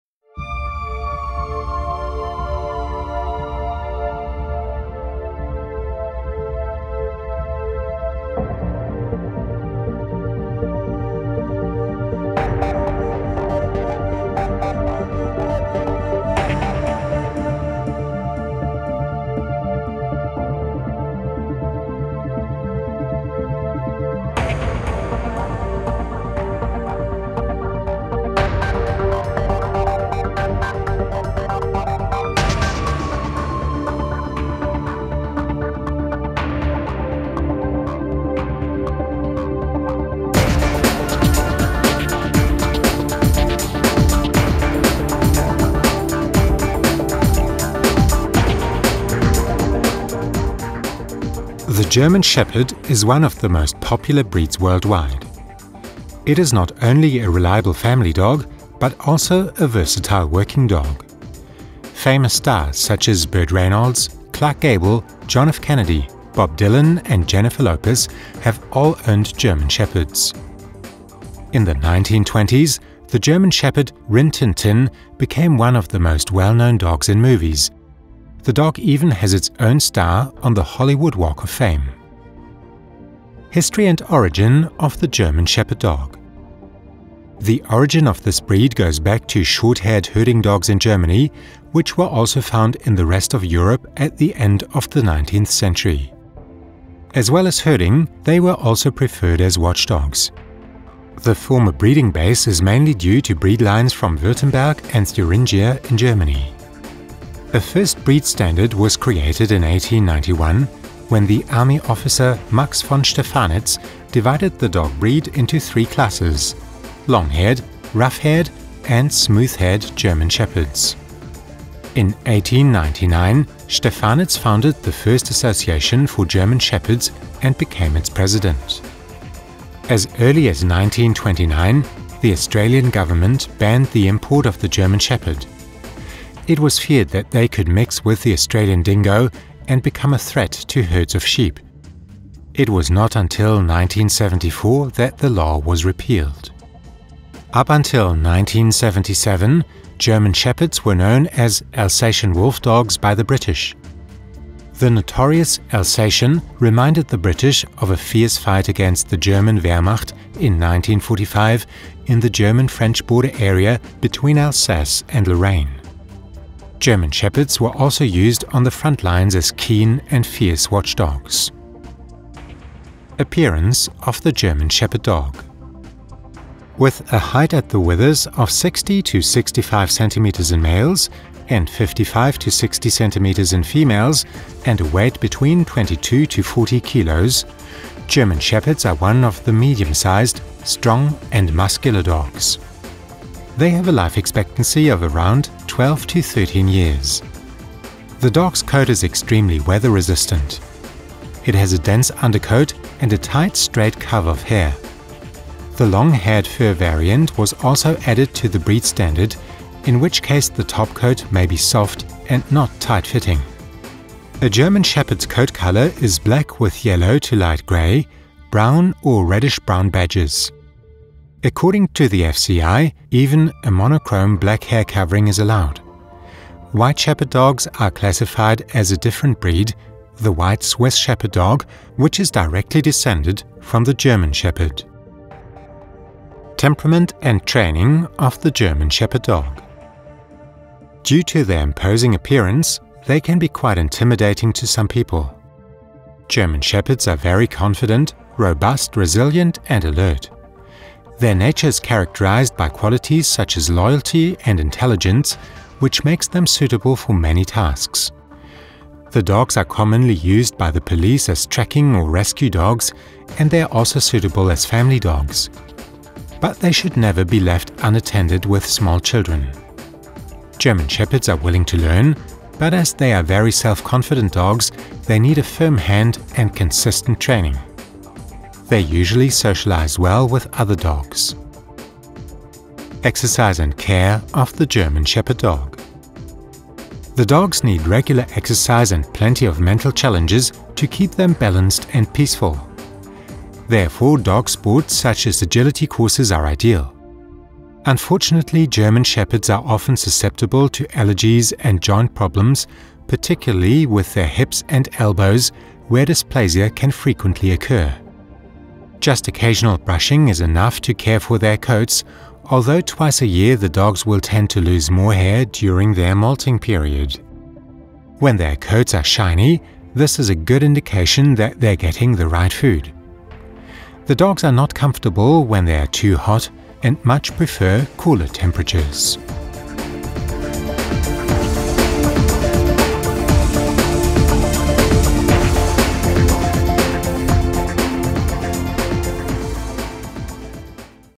► German Shepherd Dog Breed [2020] Temperament & Training
In this breed profile you will find all the important facts about the German Shepherd:
► Intro 0:00
► German Shepherd History & Origin 1:23
► German Shepherd Appearance 2:52
► German Shepherd Temperament & Training 3:56
► German Shepherd Exercise, Care & Health Problems 4:42

German Shepherd Dog Breed Profile Training Guide
Cool German Shepherd Dog Hoodie
Funny German Shepherd Dog Mug